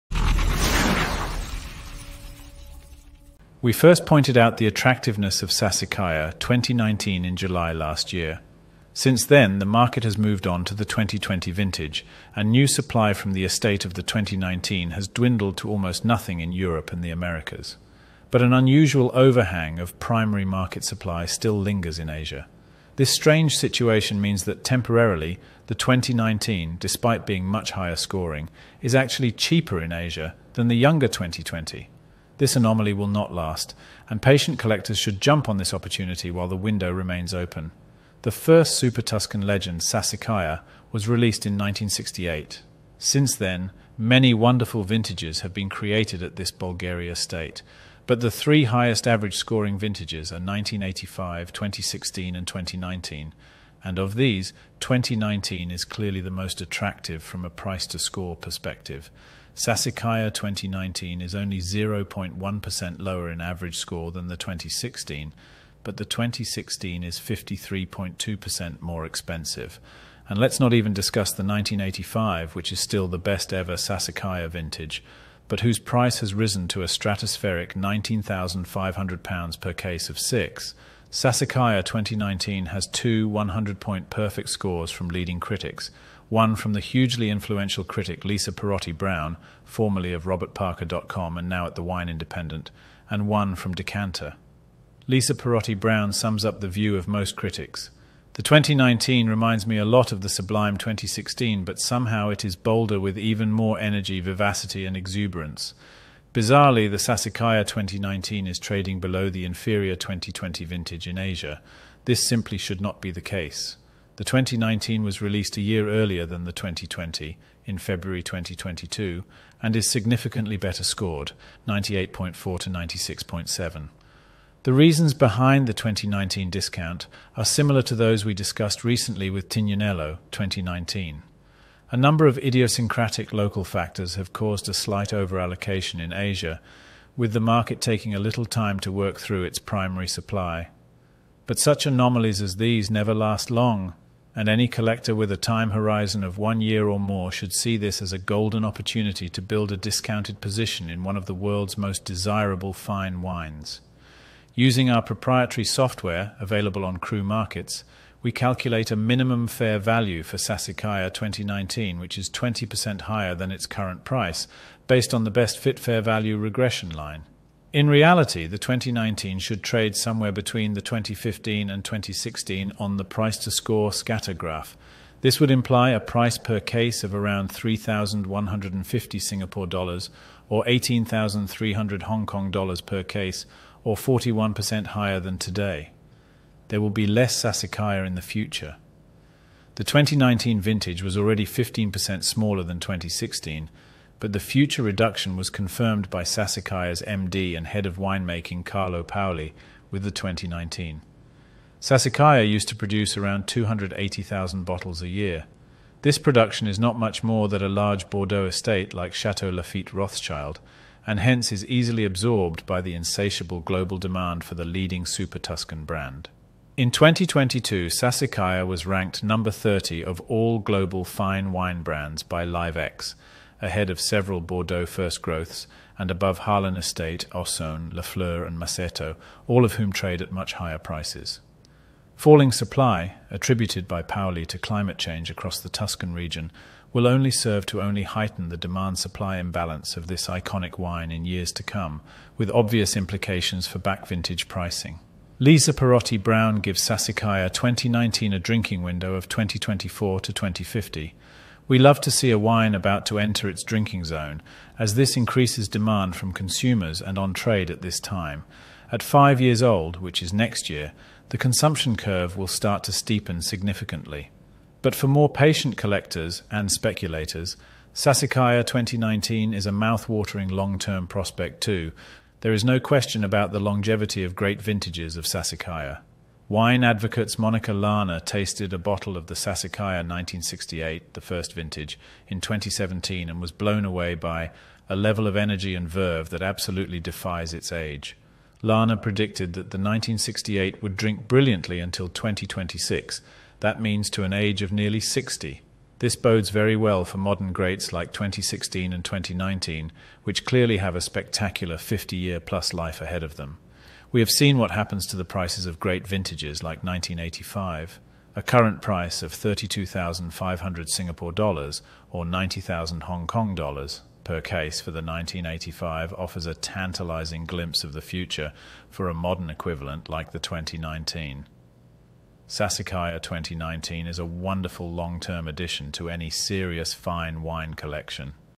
Sassicaia 2019 - 'Last Call' for Primary Market Discount
Sassicaia 2019 is a fabulous long-term holding for all fine wine collections.

Right now, there is an unusual situation where the Sassicaia 2019 is trading too low versus the 2020 (and other vintages) due to a primary market overhang in Asia,

This situation will not persist, and we see 20-40% upside potential for Sassicaia 2019 over a 1-3 year holding period.